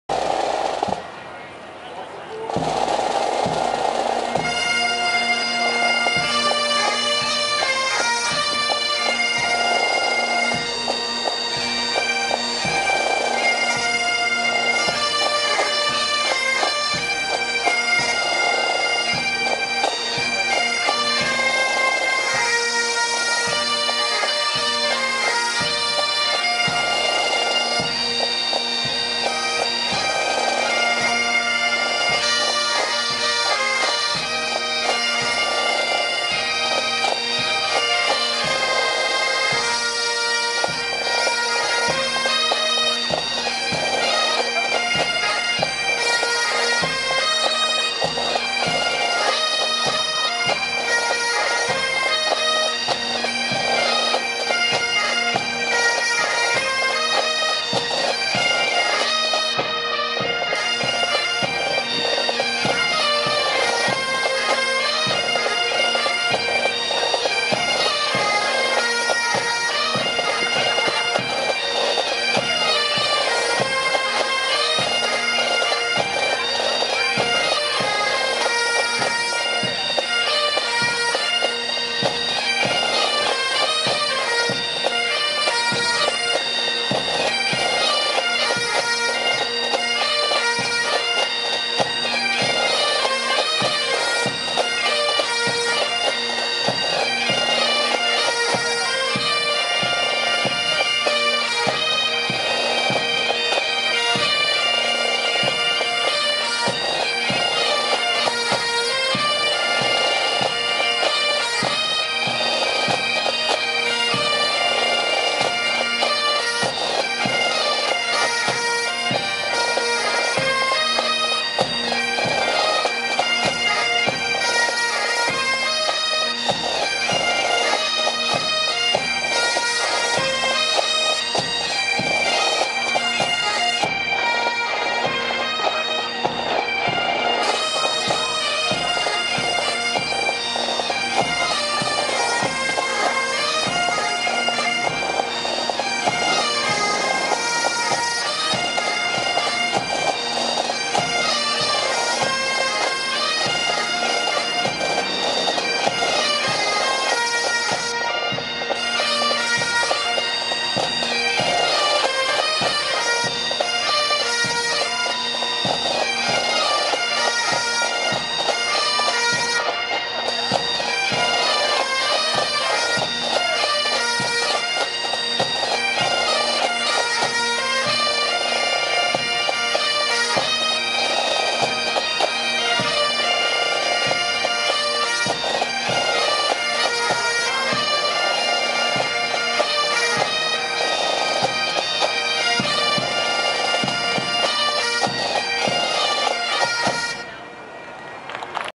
NHHG - Drum Majors competition.MP4
New Hampshire Highland Games, Drum Majors competition, September 17, 2011.  Duty band is the Royal Canadian Mounted Police Pipes and Drums.